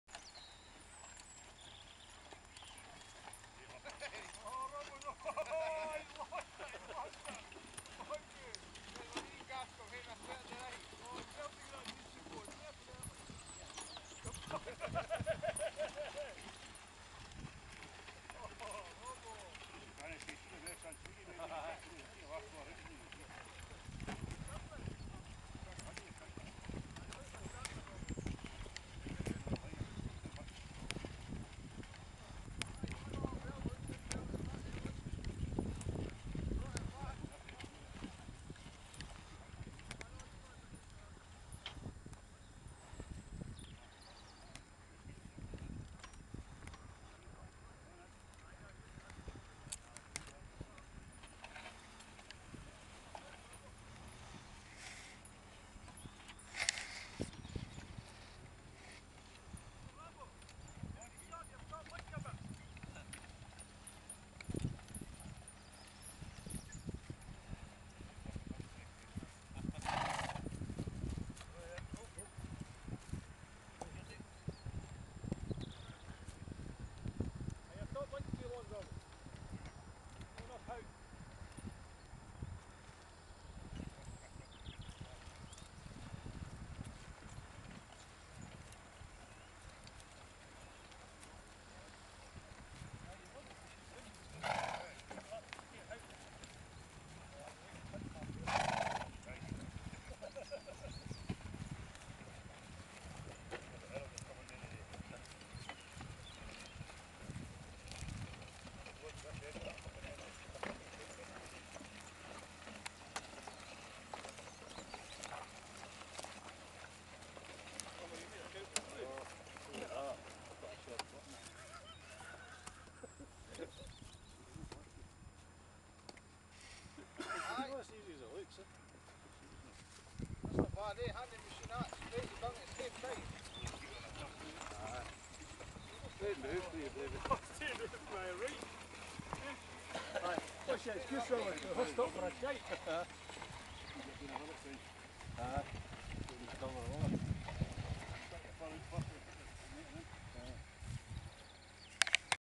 HORSE PLOUGHING
Stuart Robertson being shown by Brendan Sweeney how it used to be. NOT EASY!!!